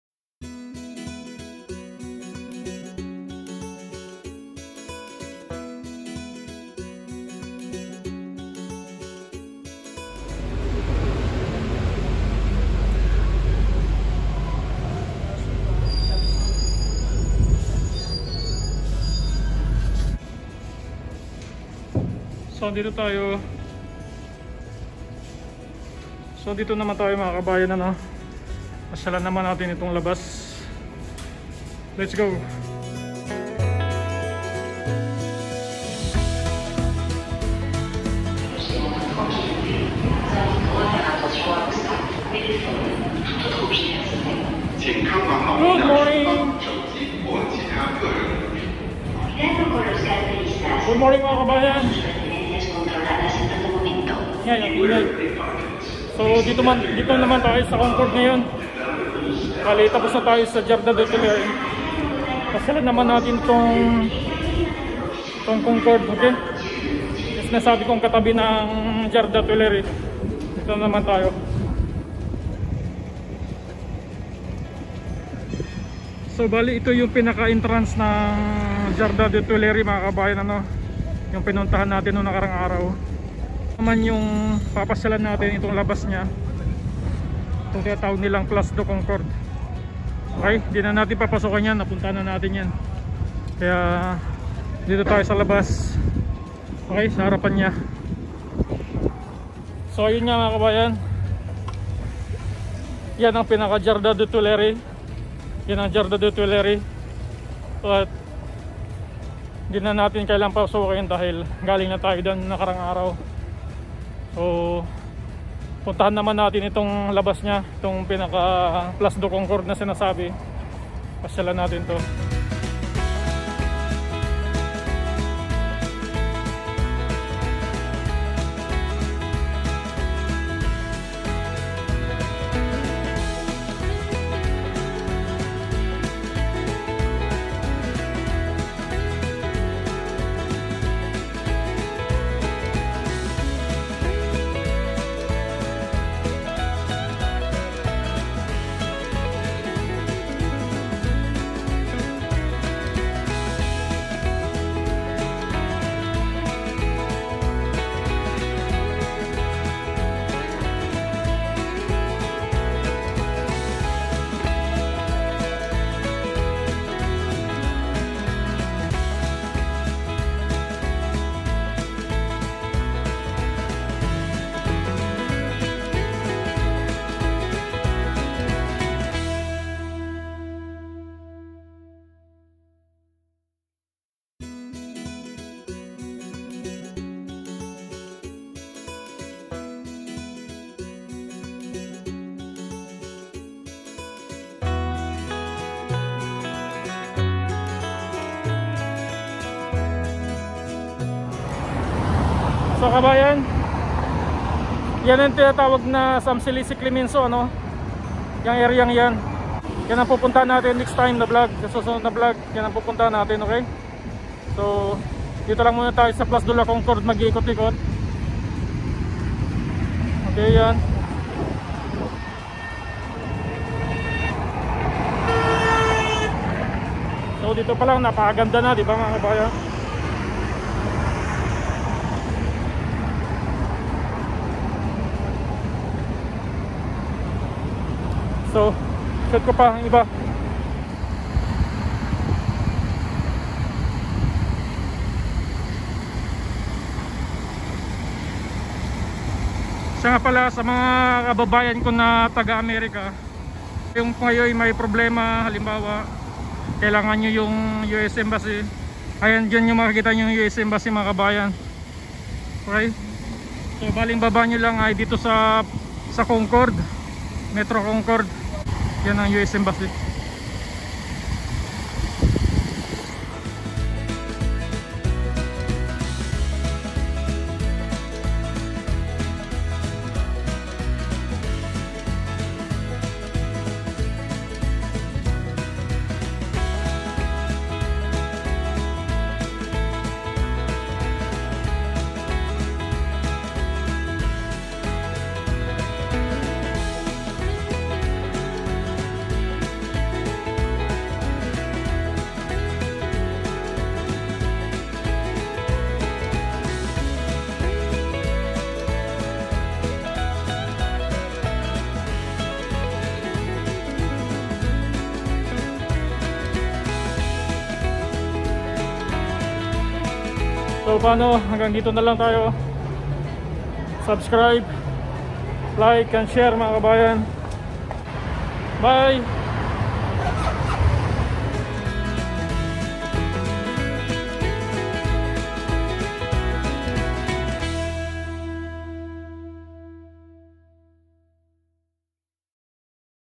Paris vlog.place de la Concorde
Samahan niyo ulit ako mamasyal mga kabayan dito sa  Paris. place de la Concorde.               # Pranseng Hilaw #